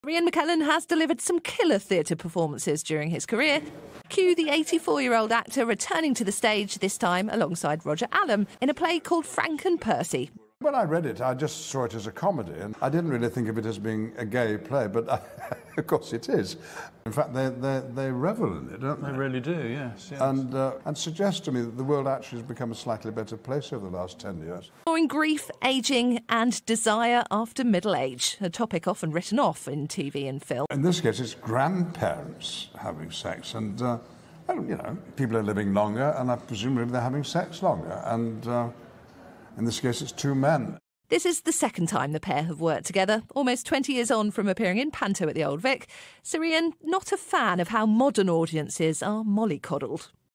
Sir Ian McKellen Takes the Stage for a Hilarious and Unapologetically Gay Comedy
🎭 "Sir Ian McKellen & Roger Allam: Unapologetically Gay Comedy Onstage 🏳️‍🌈😂

Prepare for a laughter-filled night as Sir Ian McKellen and Roger Allam bring their unapologetically gay comedy to the stage. This video captures the essence of their hilarious performance and celebrates LGBTQ+ comedy in all its glory.